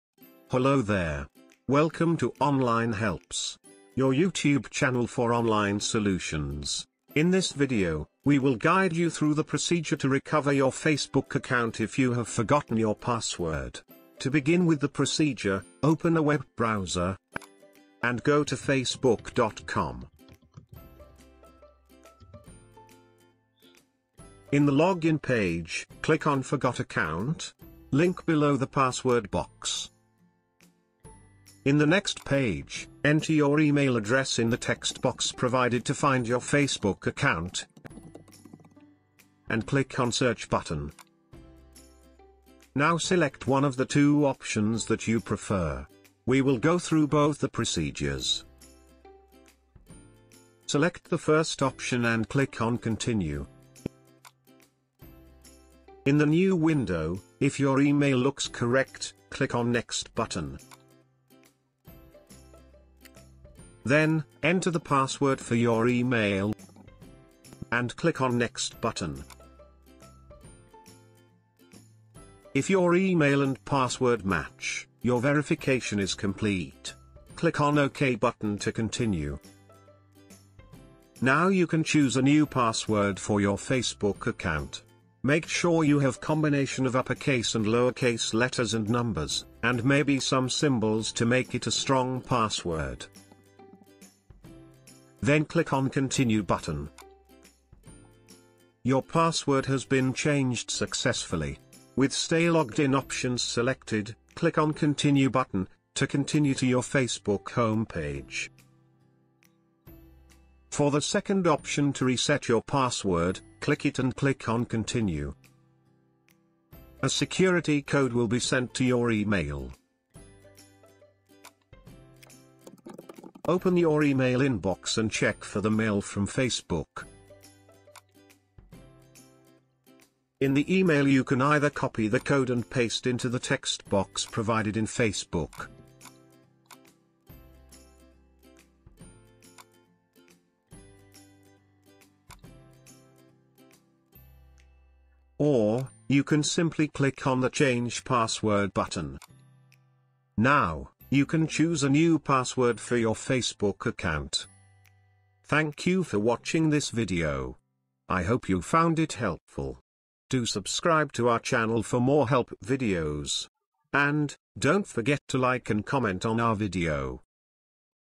How To Reset Facebook Password | Forgot Facebook Password
This video will guide you through the procedure to recover facebook account if you have forgotten your password, or security of your account has been compromised and you no longer have access to your account. To reset your facebook password:

2. Click on "Forgot Password?" link below password box.
3. Enter your email and click on "Continue".
4. Choose one of the two options to recover using your email.
5. a. For first option: login to your email using the password for your email.
5. b. For second option: check your email inbox and use the code to get a new Facebook password.
7. Choose a new password for your account.

forgot fb password
reset fb password
reset facebook password using email